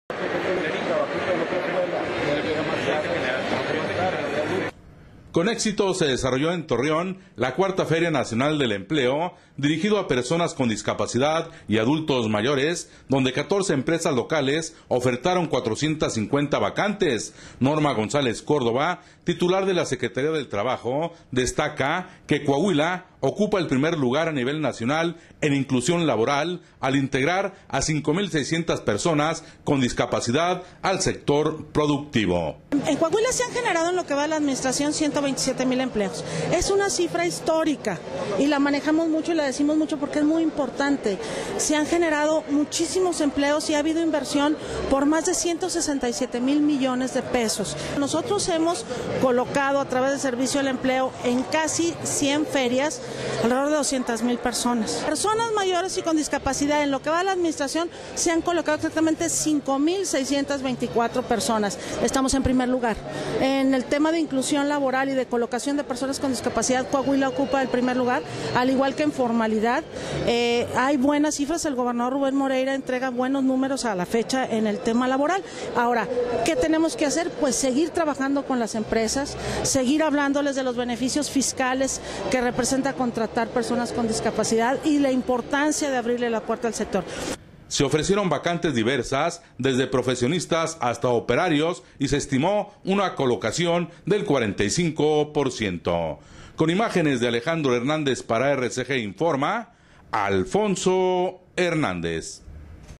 Ofrecen empleo a personas con discapacidad
Torreón, Coah; Con éxito se desarrolló en Torreón la Cuarta  Feria Nacional del Empleo  dirigida a personas con discapacidad y adultos mayores, donde 14 empresas locales ofertaron 450 vacantes.

Norma González Córdova, titular de la Secretaría del Trabajo, destaca que Coahuila ocupa el primer lugar a nivel nacional en inclusión laboral al integrar a 5 mil 600 personas con discapacidad al sector productivo.